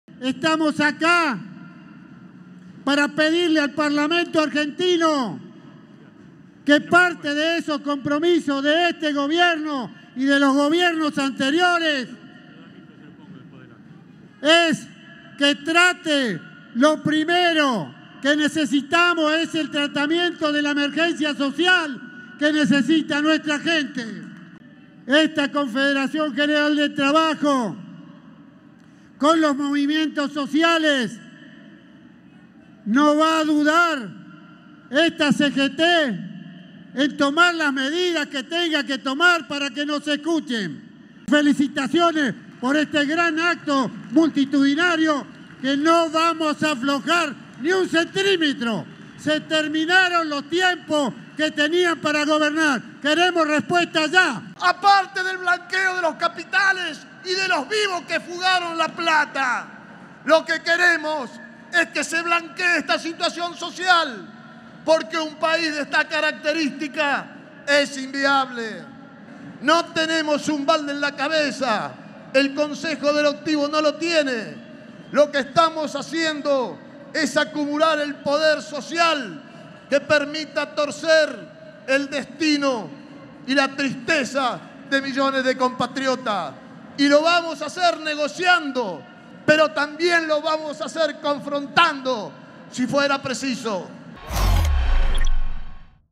Acuña advirtió que se "terminó el tiempo de espera" y reclamó "respuestas concretas" al Gobierno
Carlos Acuña, uno de los tres titulares de la CGT unificada, aseguró hoy que se "terminaron los tiempos de espera", reclamó al gobierno nacional que se "haga cargo" de la situación social y que dé "respuestas concretas" a los trabajadores, durante el acto de cierre de la marcha en reclamo de una ley de emergencia social.
Desde el palco montado a espaldas del Congreso Nacional, Acuña aseguró que la CGT "no va a dudar en tomar las medidas que sean necesarias" para obtener la emergencia social porque "se terminaron los tiempos" de espera.
Acuña no descartó seguir conversando con el Gobierno pero advirtió que "si no hay respuesta esas conversaciones son inútiles".
"Estamos dando un mensaje de unidad para tratar de que este parlamento escuche y de respuestas", sostuvo Acuña en el acto convocado por la CGT y organizaciones y avisó que esa central sindical "va a estar atenta a que el gobierno cumpla con los compromisos de campaña".
Por último, el titular del Sindicato de Empleados de Estaciones de Servicio, Garajes y playas de Estacionamiento pidió además que se "escuche" a los trabajadores y que el "Ejecutivo se haga cargo" de la situación social y empiece a dar respuestas".